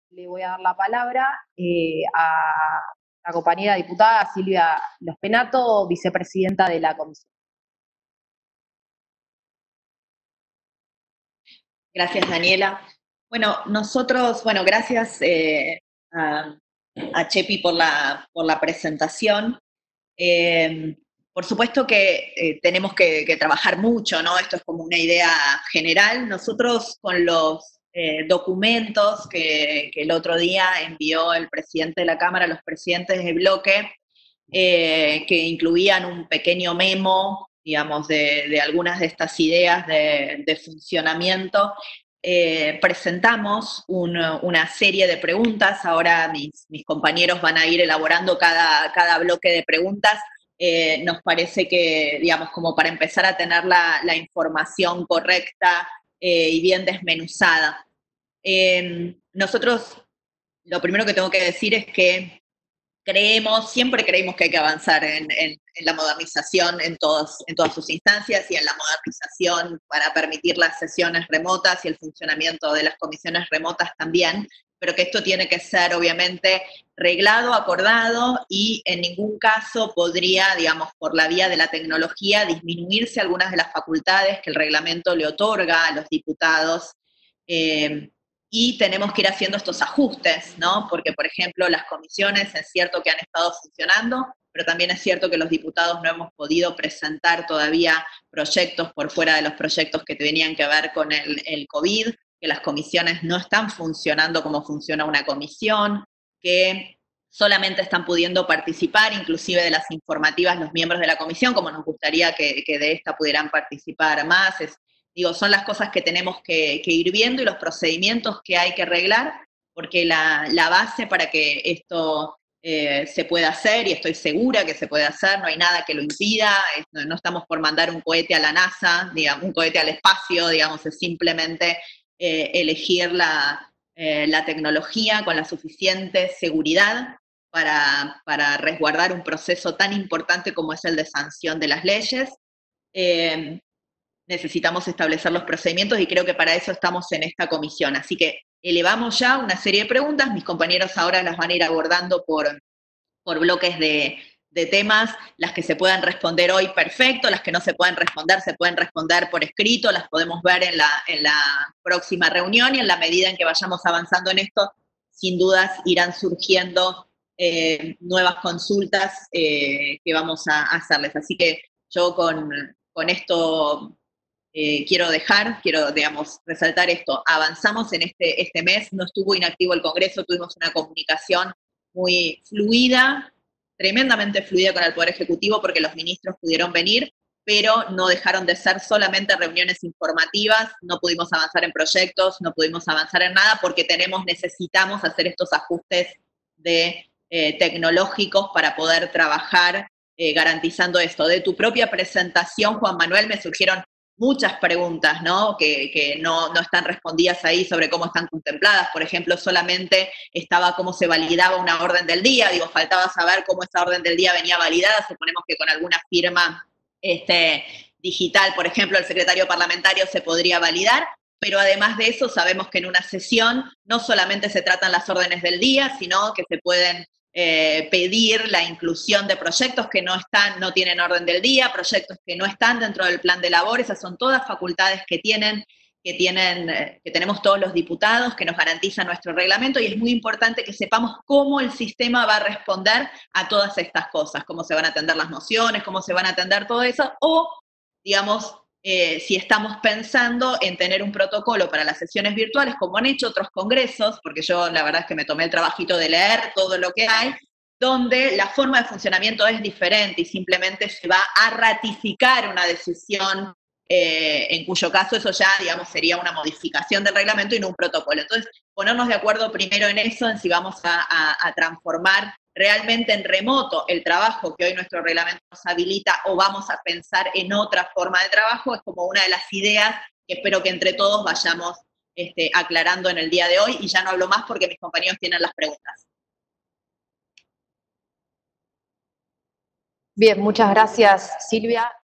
Diputada Lospennato, Silvia - Comisión de Modernización - 23-04-2020
Reunión Constitutiva por videoconferencia de la Comisión de Modernización.